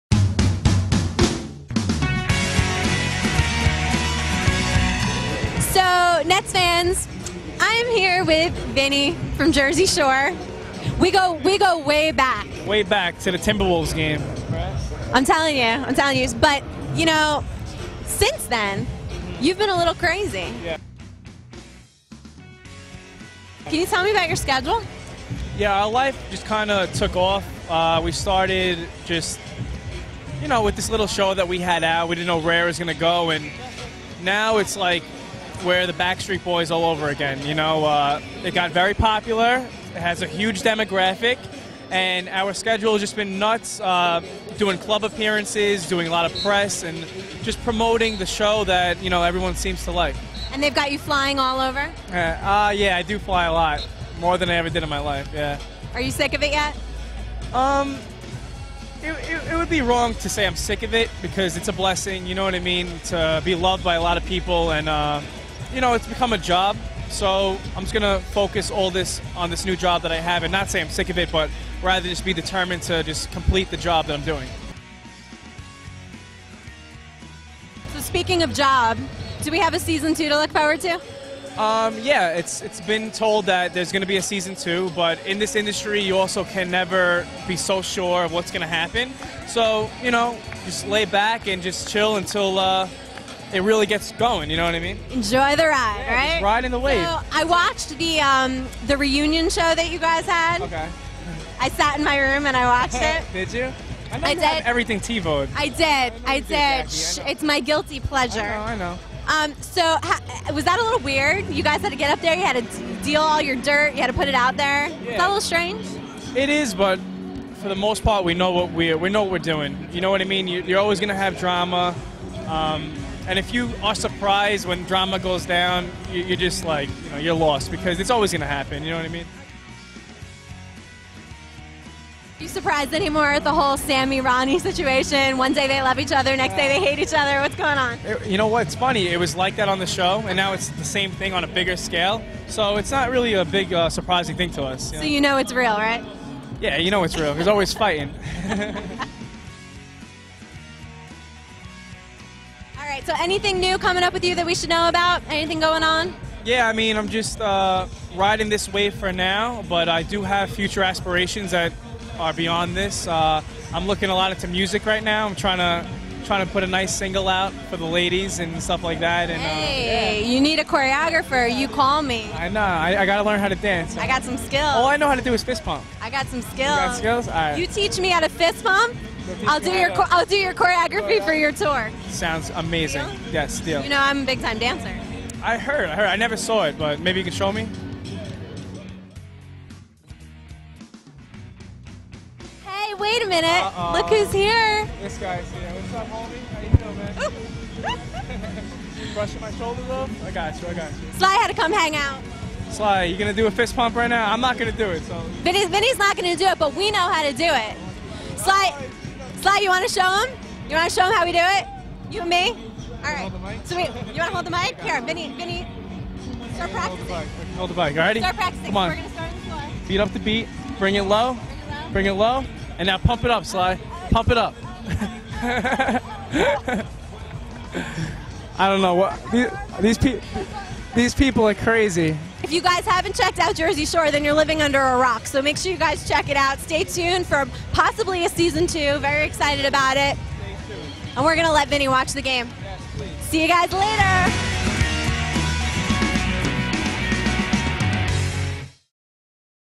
Jac of all Nets - Vinny Jersey Shore
Jaclyn Sabol interviews Vinny from MTV's 'Jersey Shore' prior to the second season, and even gets some courtside fist-pump lessons.